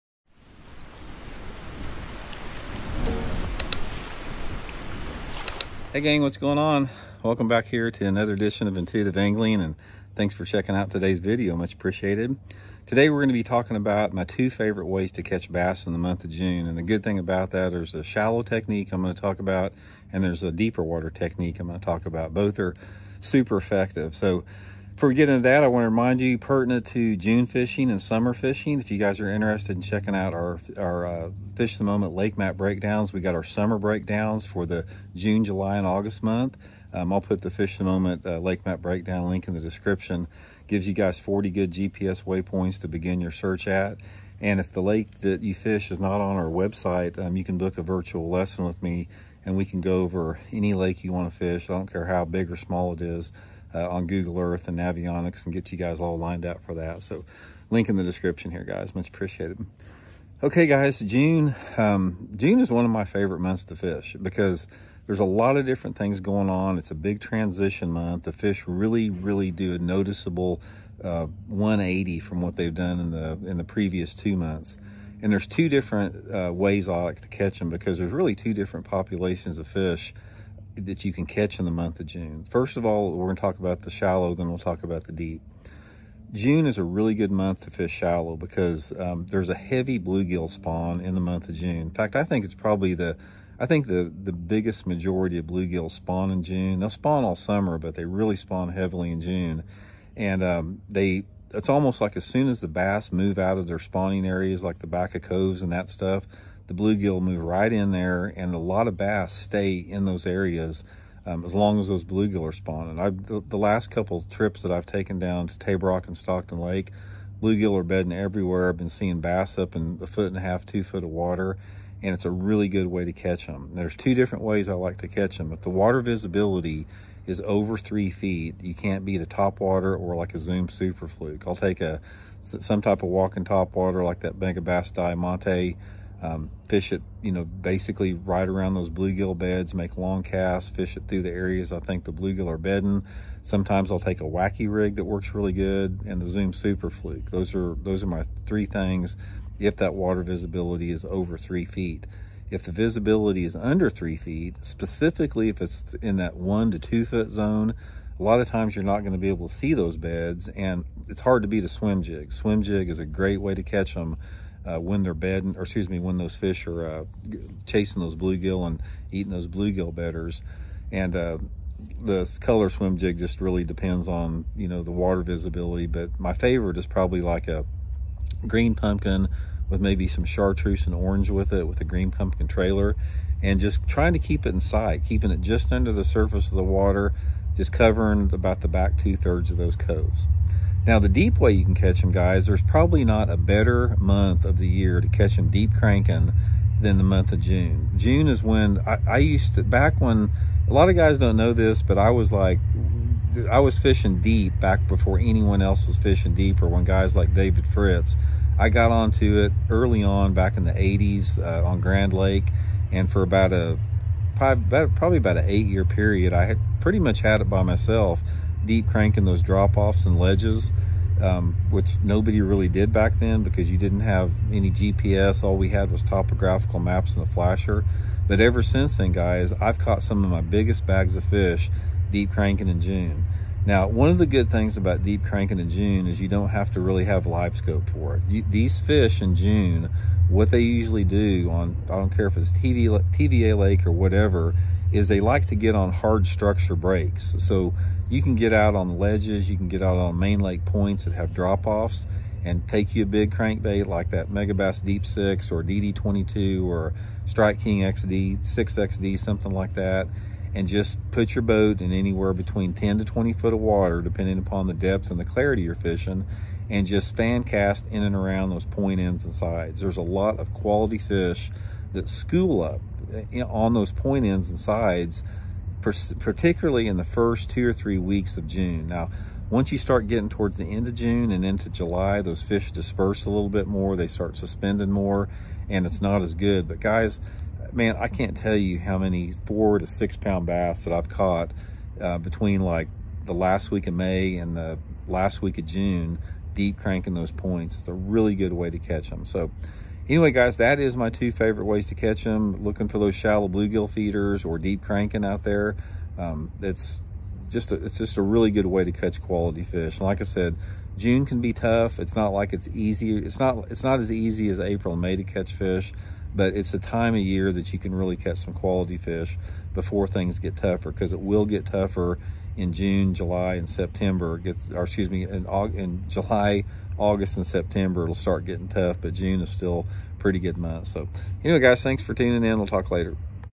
I’ve Caught Hundreds Of Big Bass In June Doing THIS…
Randy talks about the top ways to catch both deep and shallow bass in the month of June…fishingdaily